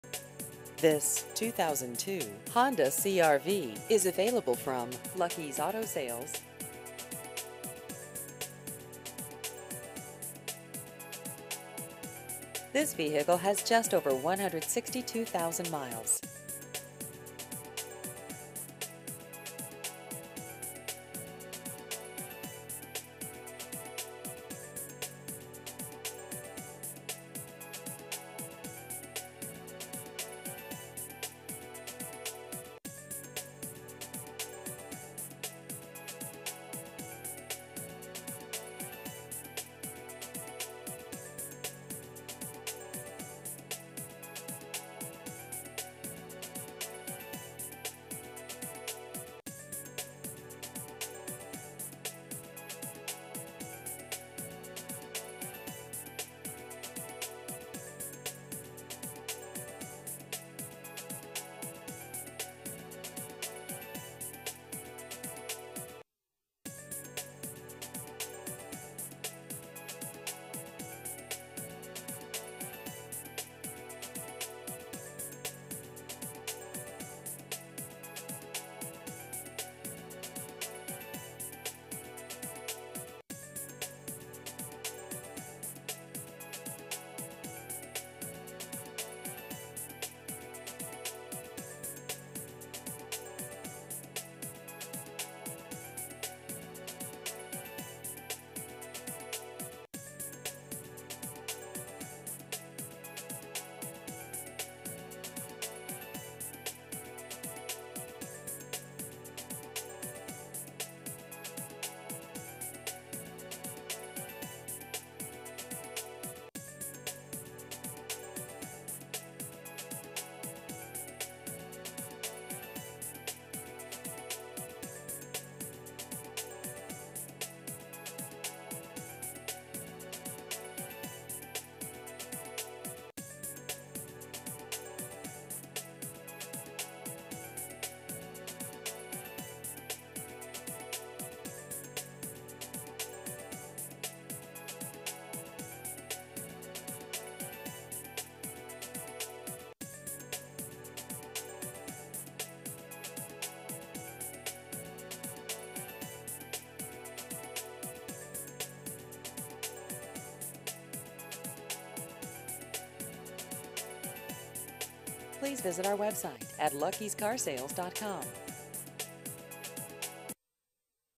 2002 Honda CR-V Used Cars Painted Post NY
This 2002 Honda CR-V is available from Lucky's Auto Sales.